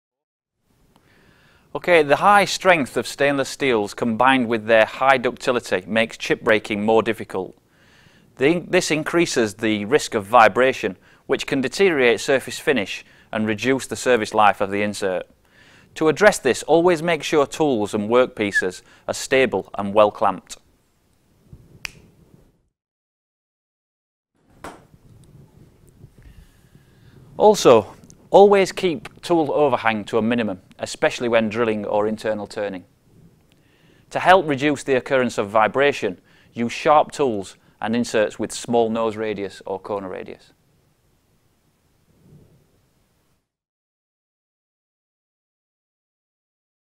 Stainless Steel - mechanical properties
Ricky Payling, application specialist at Dormer Pramet, reviews the mechanical properties of stainless steel and gives advice on machining the material.

The high strength of stainless steels combined with their high ductility makes chip breaking more difficult. This increases the risk of vibration, which can affect surface finish quality and reduce the service life of the tool or insert.

To address this, always make sure tooling and workpieces are stable and well clamped. Always keep tool overhang to a minimum, especially when drilling or internal turning.

To help reduce vibration, use sharp tools or inserts with a small nose or corner radius.